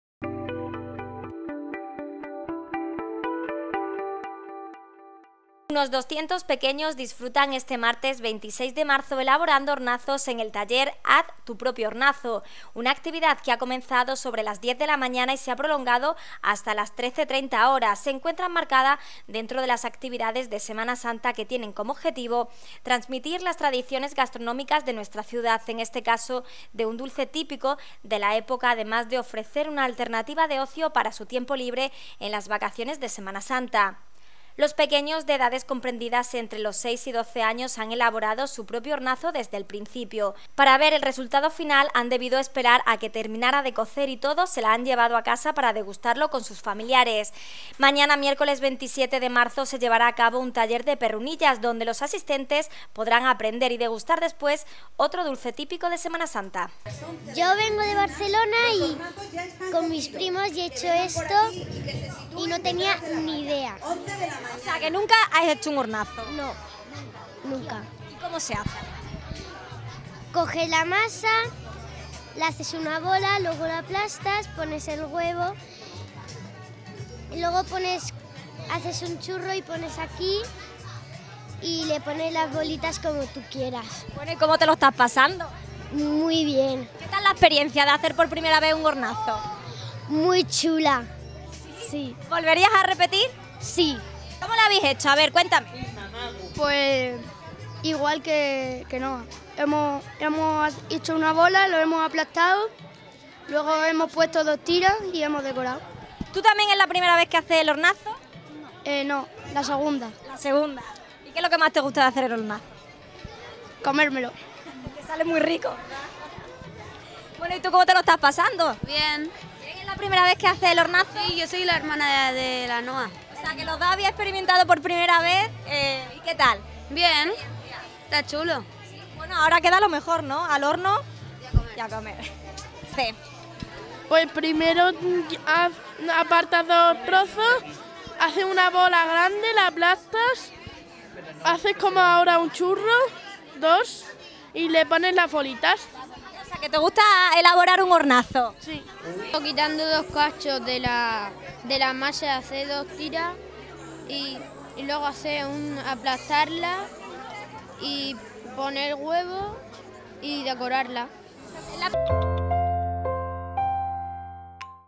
Unos 200 pequeños disfrutan elaborando hornazos en el taller “Haz tu propio hornazo”
😜 Mañana se llevará a cabo el taller de perrunillas también destinado a los más pequeños
Unos 200 pequeños disfrutan este martes 26 de marzo elaborando hornazos en el taller “Haz tu propio hornazo”. Una actividad que ha comenzado a las diez de la mañana y se ha prolongado hasta las 13:30 horas, se encuentra enmarcada dentro de las actividades de Semana Santa que tienen como objetivo, transmitir las tradiciones gastronómicas de nuestra ciudad, en este caso de un dulce típico de la época además de ofrecer una alternativa de ocio para su tiempo libre en las vacaciones de Semana Santa.